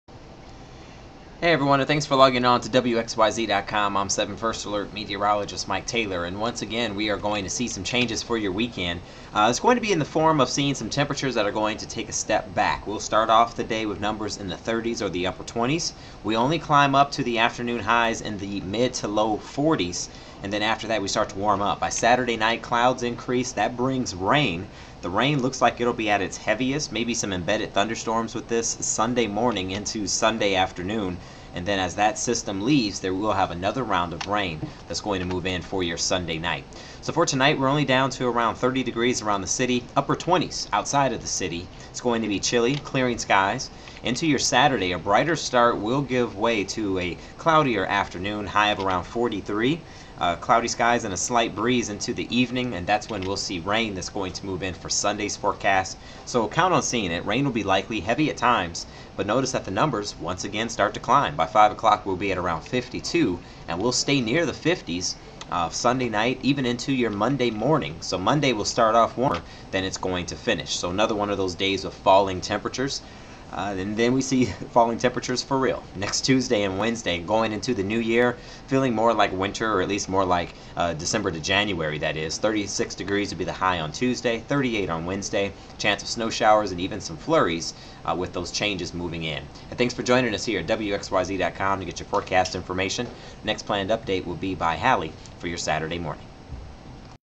Weekend Forecast
The weekend is in sight and another warm up is on the way. 7 First Alert meteorologist Mike Taylor has the latest.